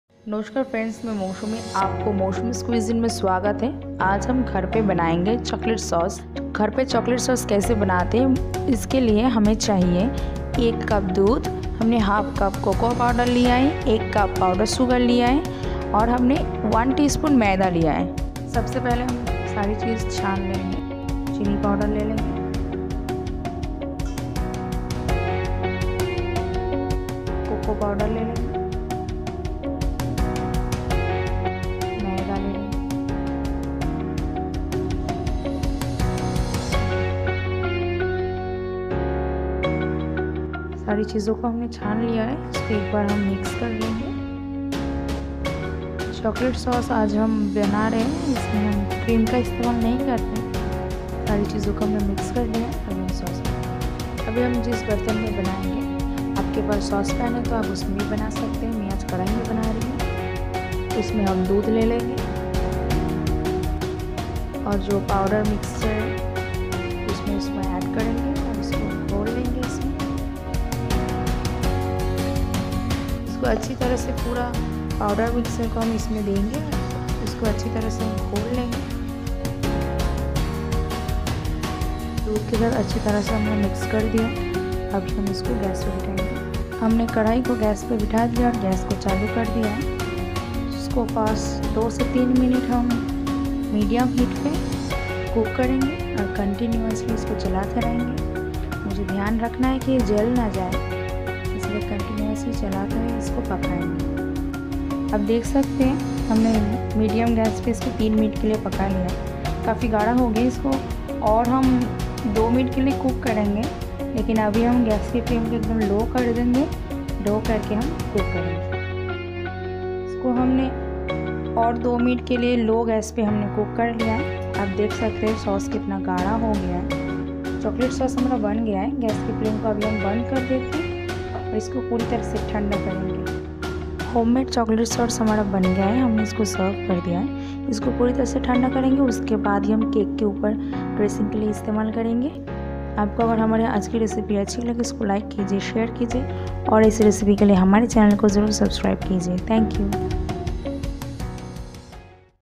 Home made Chocolate Sauce without butter,cream | Chocolate Syrup Recipe | चॉकलेट सिरप बनाने की विधि
# Home made Chocolate Sauce # How to make Chocolate Sauce # Chocolate Syrup Recipe# Chocolate Sauce without Cream, butter#Chocolate Sauce use Cocoa Powder

Ingredients

Milk:1 Cup
Powder Sugar:1 Cup
Refine Flour /Maida:1tsp
Cocoa Powder:1/2 Cup